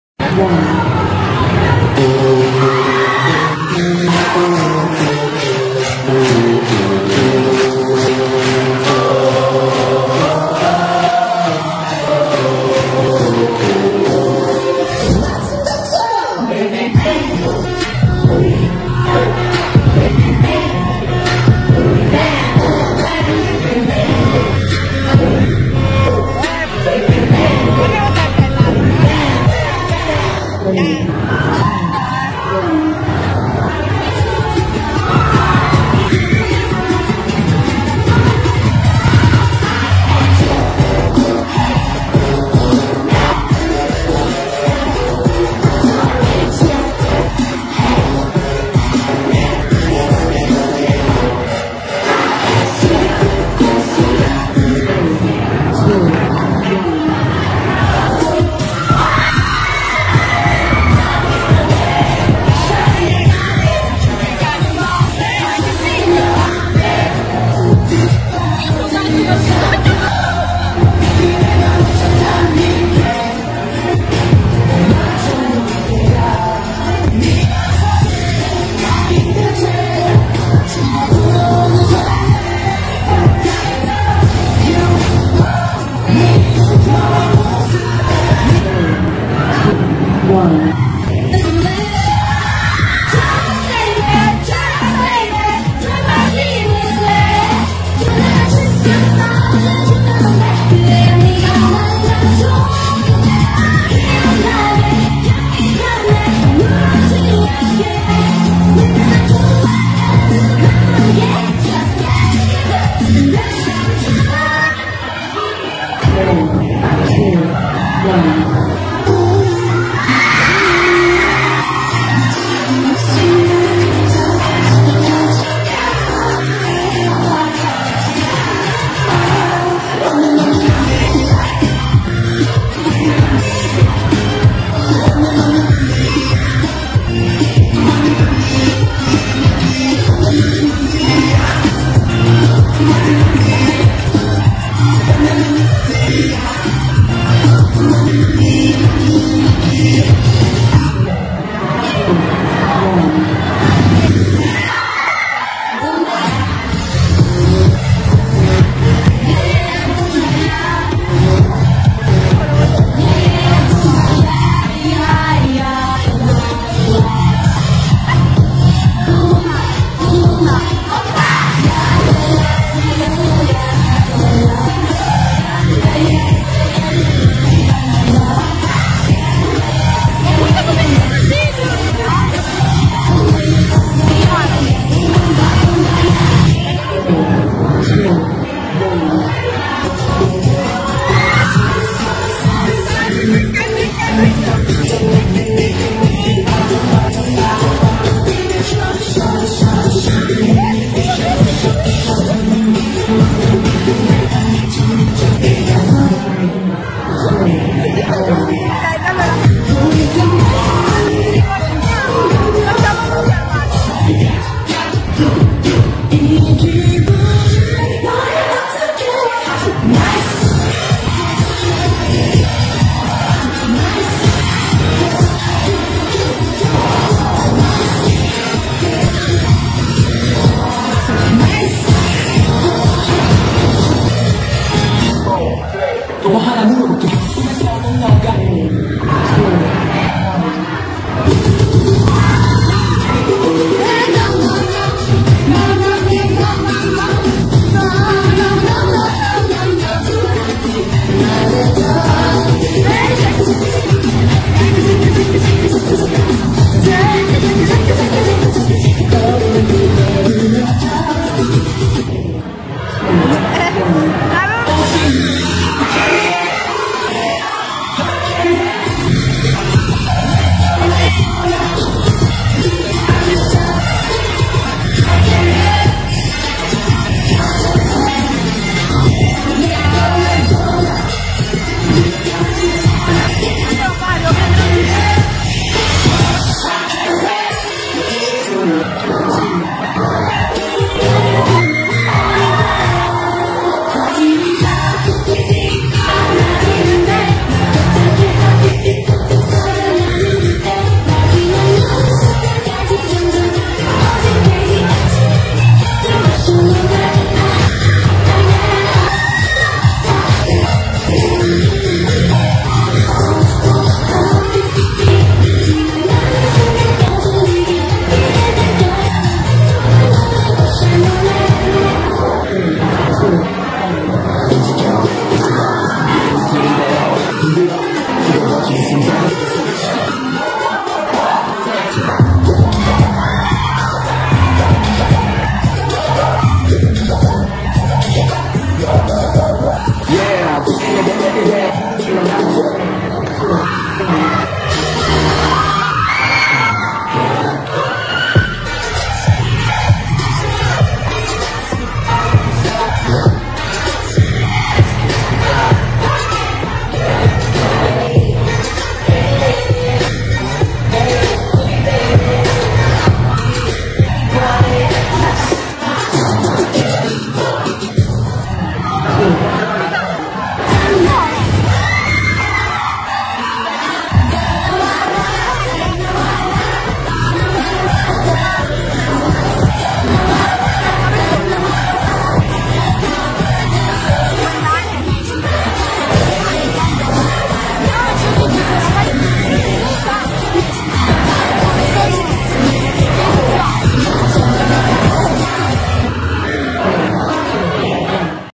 ฝรั่งต่างชาติยุคหลังๆเเอนตี้K-POPรุนแรง..!?ชาวยุโรปสงสัยชาวเอเชียชอบไปได้อย่างไร
😍เมื่อฝรั่งโดน K-POP ล้างสมอง555+😂😆‼️
"💥123"ใครเต้นได้ออกมาแหม่เก็บได้ทุกเพลง🎼